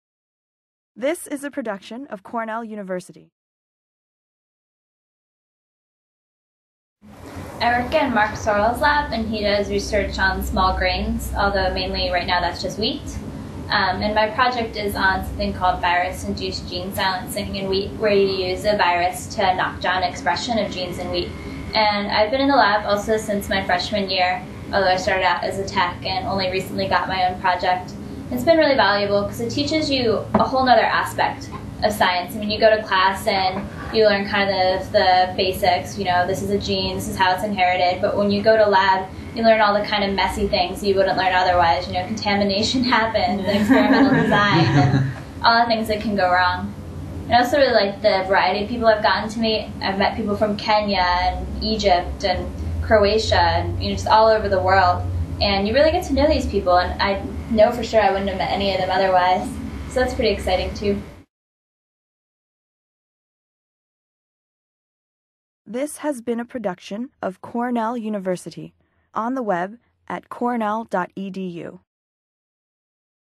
Hands on science - Plant Sciences undergrad program at Cornell University
Students discuss their experiences as undergraduate Plant Sciences majors at Cornell University's College of Agriculture and Life Sciences.